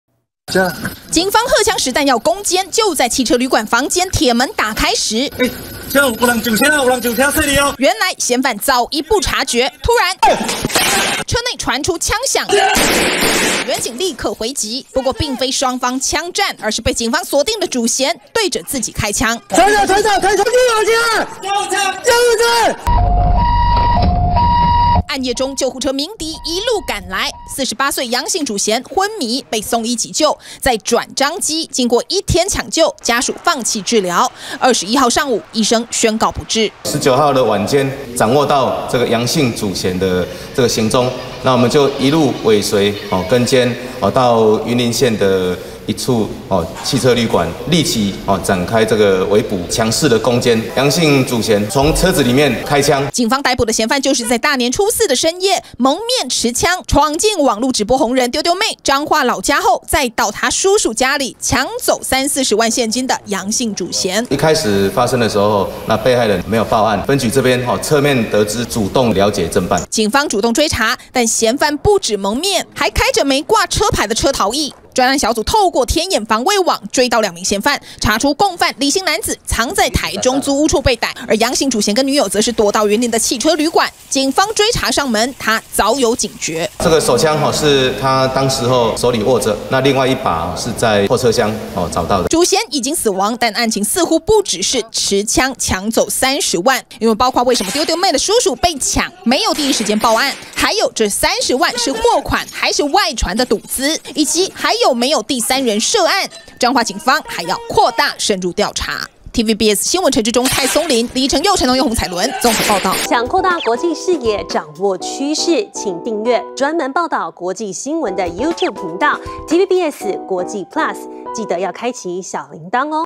「丟丟妹」叔遭搶! 主嫌躲摩鐵警攻堅 自轟不治｜TVBS新聞
網路直播主"丟丟妹"的叔叔過年期間遭歹徒闖入住處，搶走30萬現金，警方掌握主嫌躲在雲林一處汽車旅館，圍捕攻堅，但楊姓主嫌卻在警方 攻堅時對自己開槍，搶救一天後，被宣告不治，另一名共犯在租屋處被逮，警方還要深入調查，為什麼大半夜搶匪會知道屋內有大筆現金，以及是否還有其他人涉案!